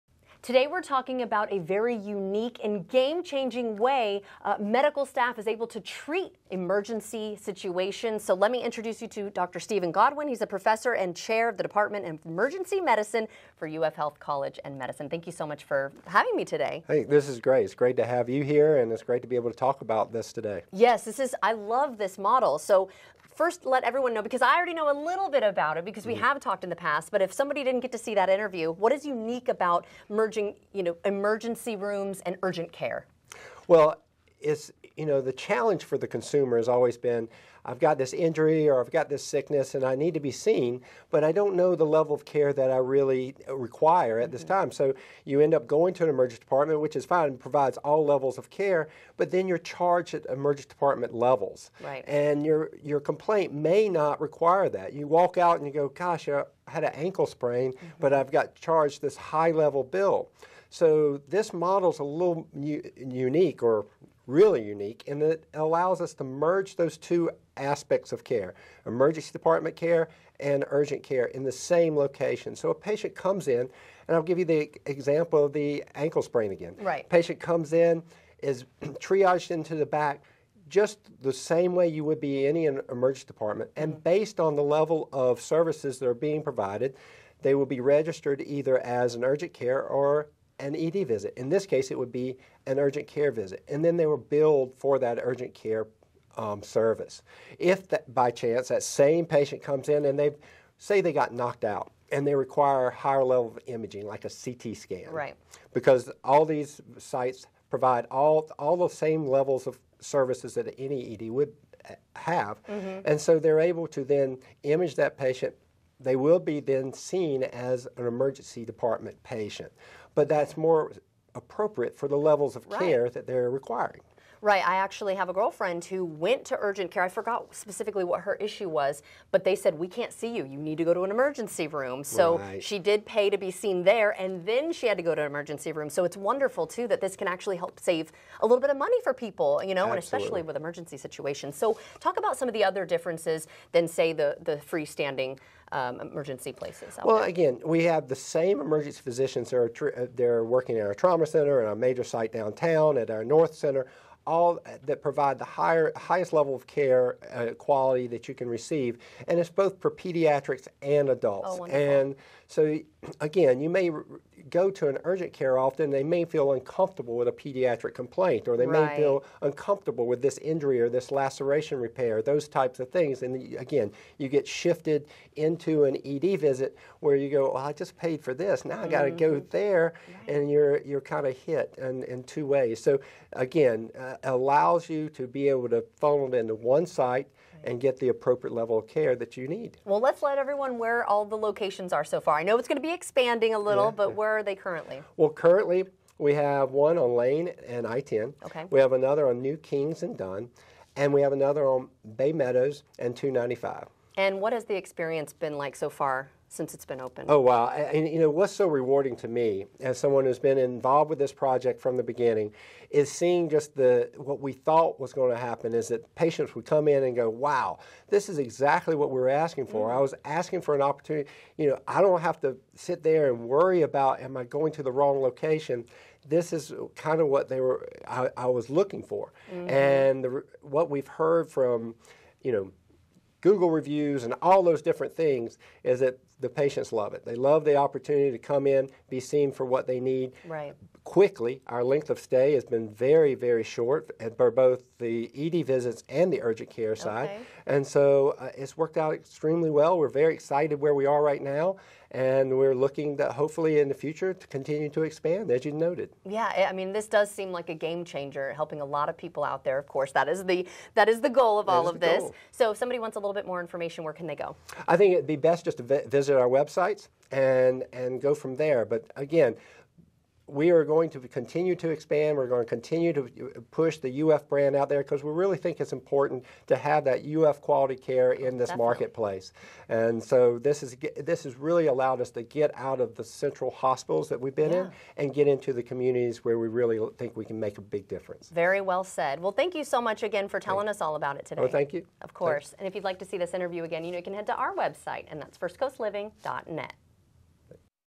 UF Health Jacksonville opens three hybrid emergency and urgent care centers
Professor and Chair of the Dept. of Emergency Medicine at UF College of Medicine, Dr. Steven A. Godwin shares how this merge benefits patients.